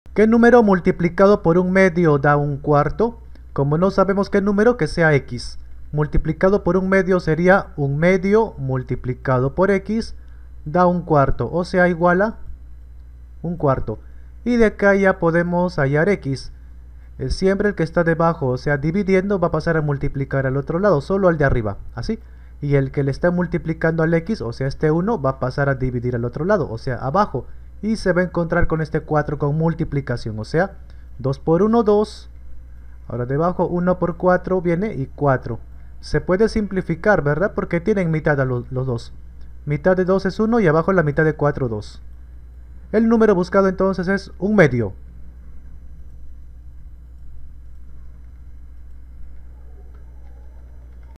Que numero multiplicado por 1/2 da 1/4 , que fraccion multiplicada por
que numero multiplicado por la fraccion da como resultado